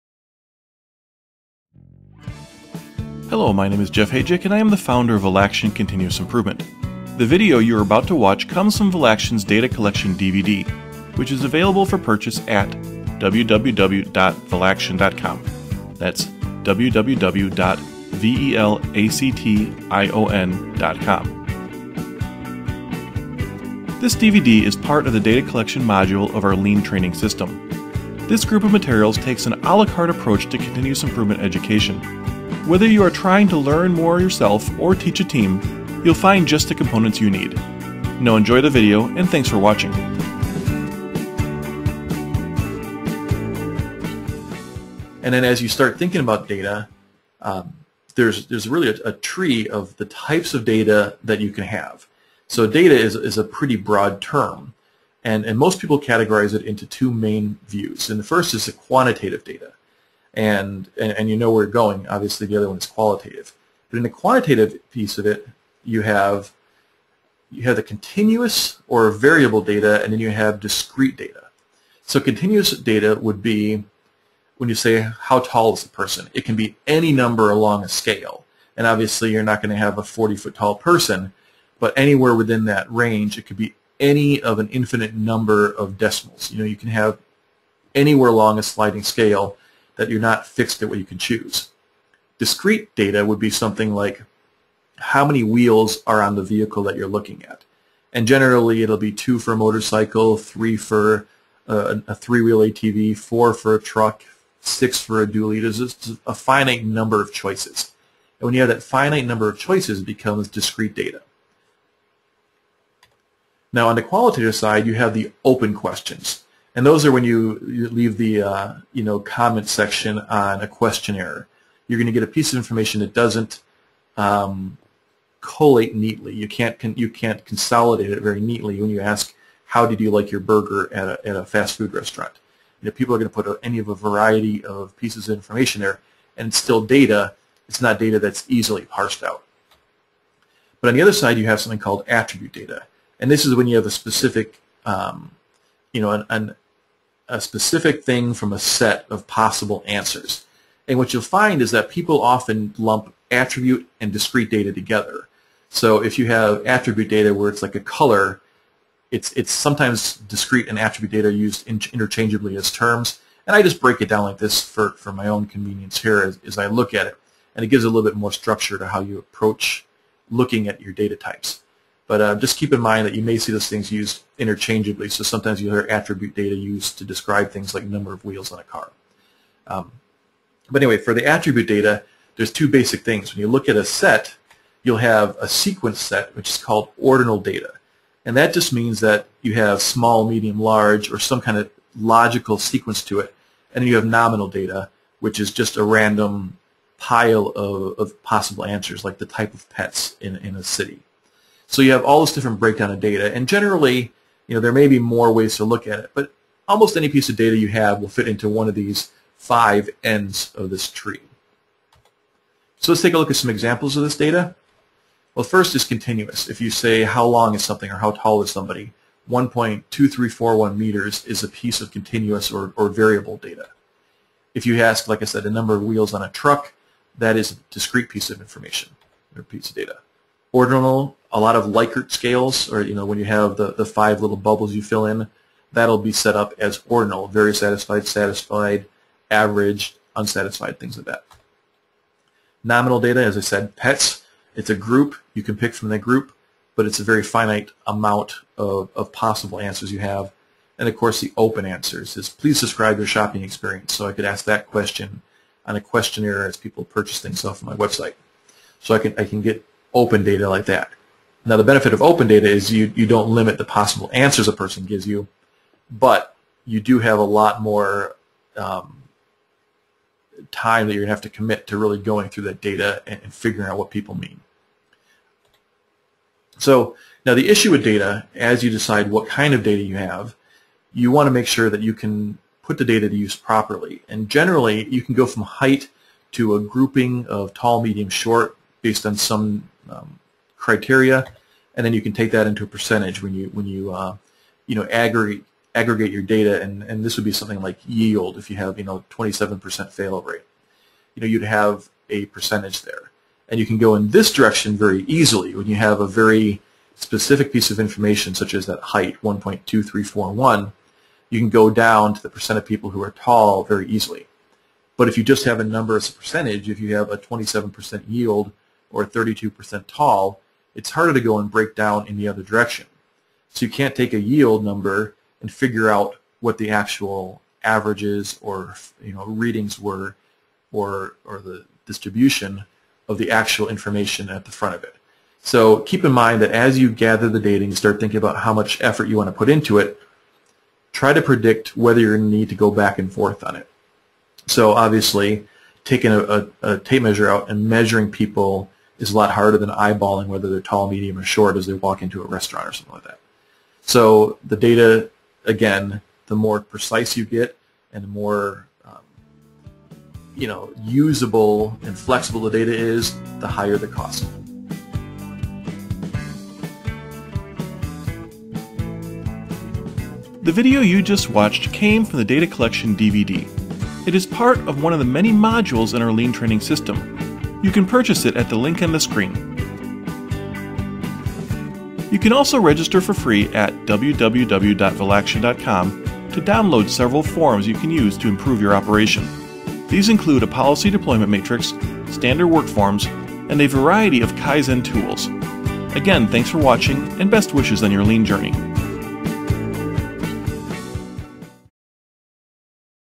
Data Collection: Understanding the Types of Data.
Data falls into several categories. Each type has some pros and cons, and is best suited for specific needs. Learn more in this short video from our Data Collection Training available